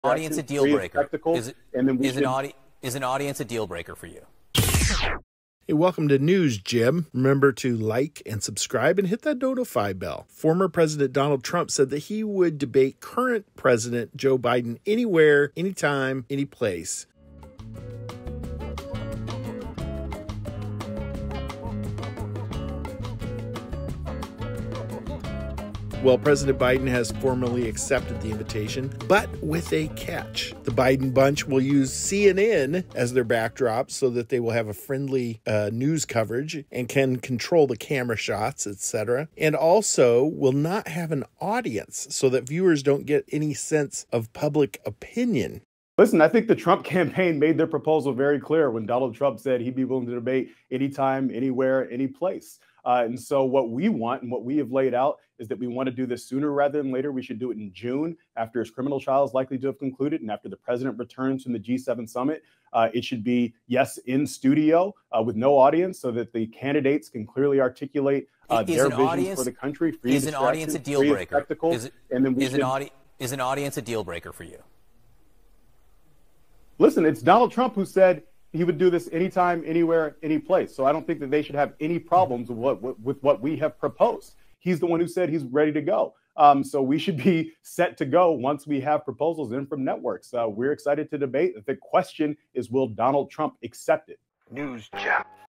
Will Debate Trump - But Demands No Audience
President Joe Biden Finally Says He Will Debate Former President Donald Trump But Only If There Is No Audience.

And click the Notify BELL to get vids first uploaded.